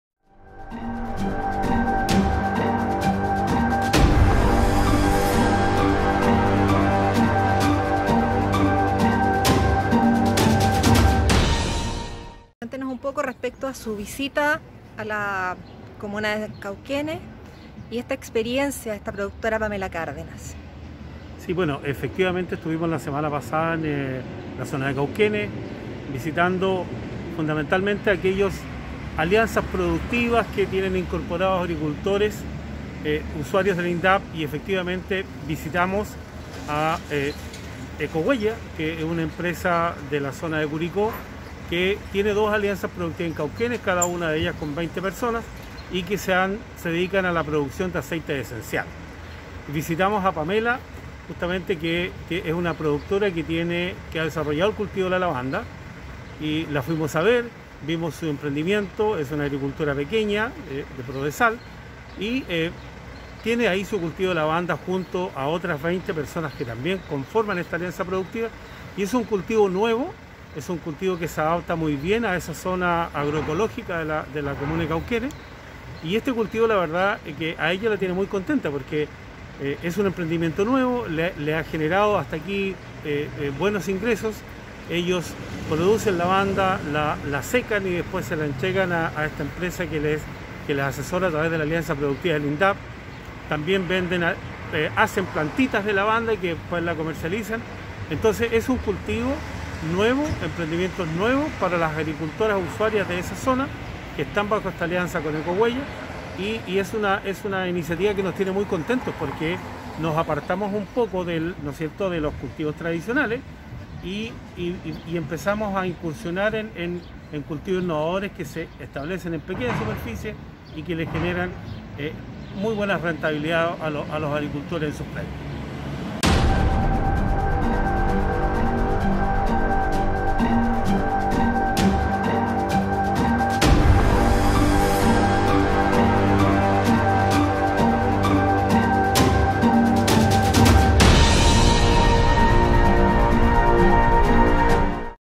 Indap Maule visita cultivo de Lavanda en Cauquenes
Desde hace tres años Pamela Cárdenas, usuaria del Programa de Desarrollo Local-PRODESAL- de INDAP, del sector Coronel de Maule, Cauquenes, incursiona en el cultivo de la lavanda, de la mano de la empresa Ecohuella, que mantiene una alianza productiva con INDAP.